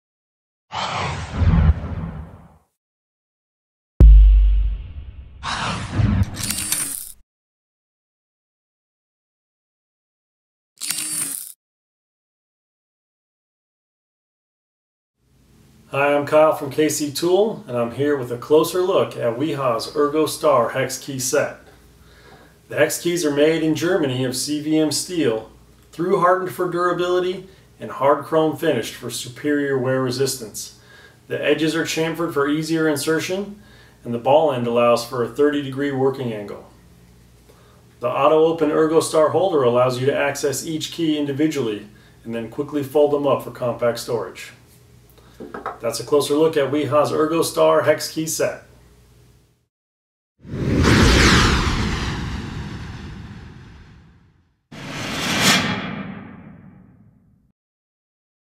Wiha ErgoStar Auto Open Hex Key Set
KC Tool is no longer a distributor for Wiha tools.

Kc Tool Provides a closer look at Wiha's German Made ErgoStar Auto Open Hex Key Set.  The Professional Quality Hex sets come in Metric and Standard. They are made of CVM Steel, Hard Chrome Finished and through hardened for durability.